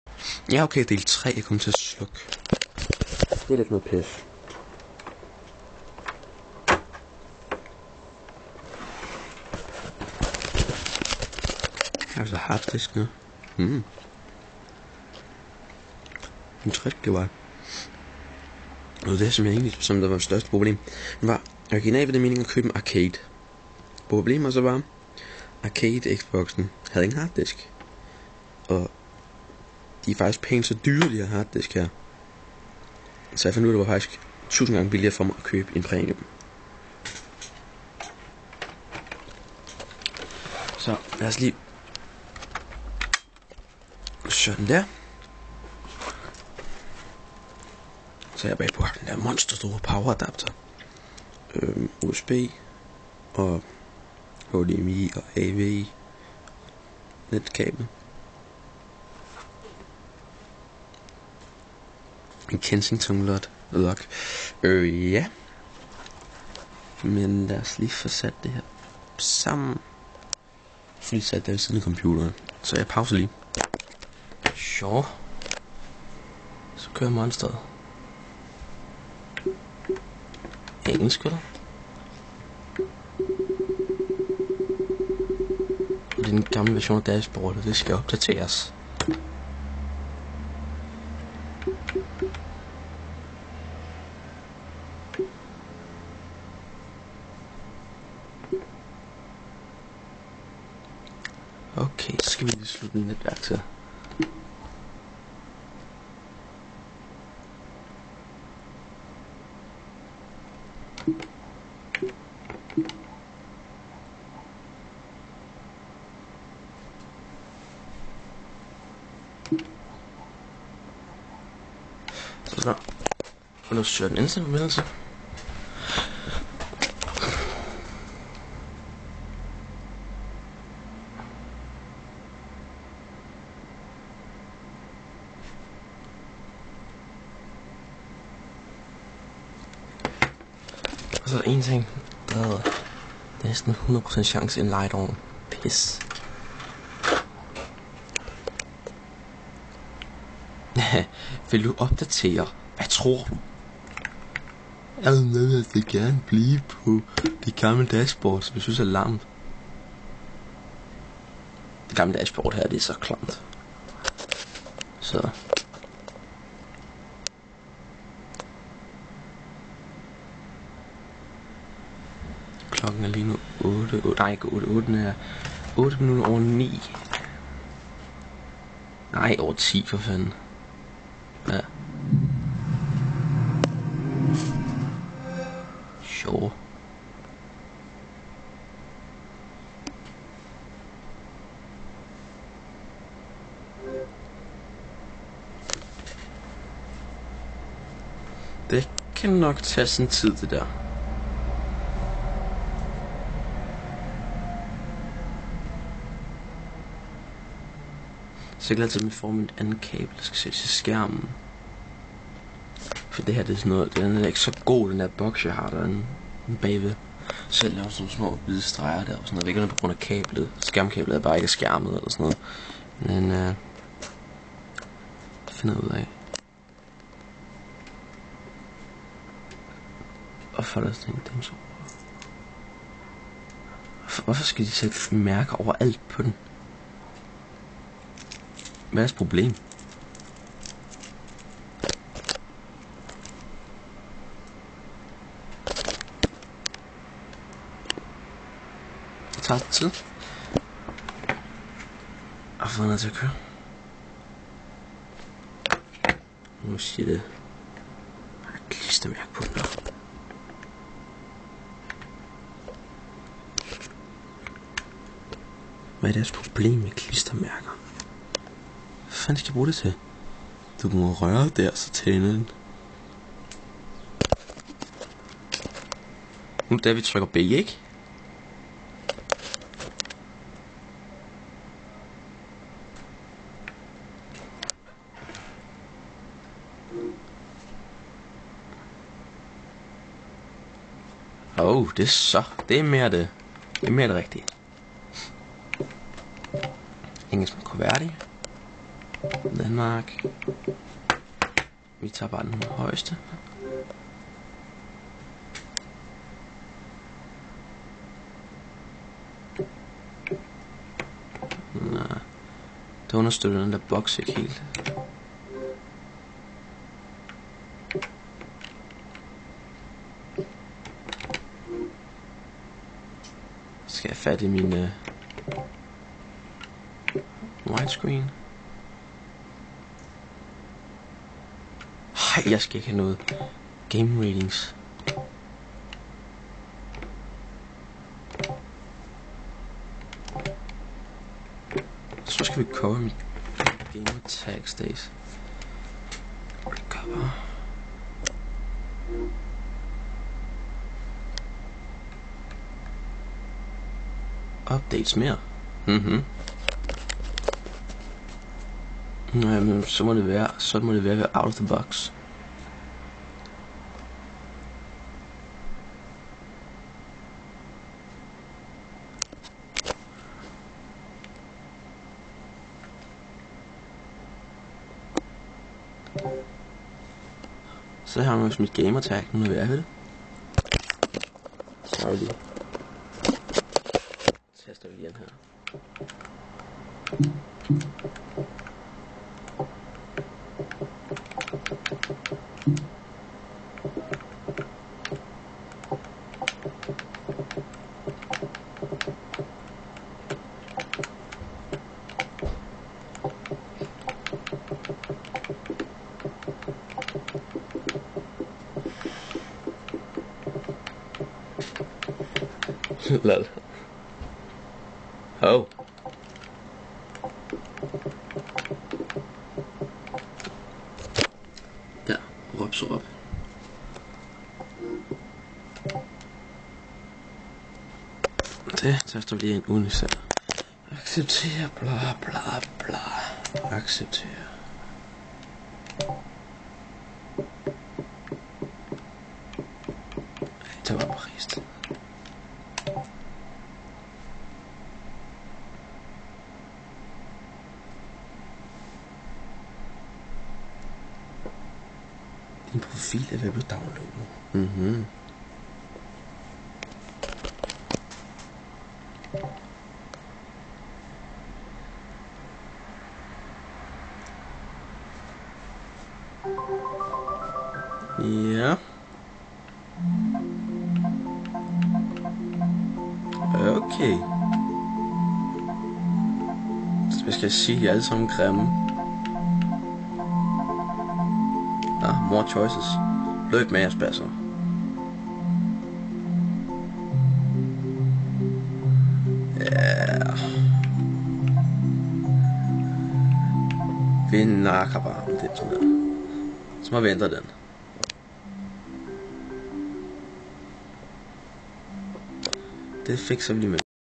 Xbox 360 Udpakning 3 ud af 3
Mig der udpakker min nye xbox 360 premium 60 GB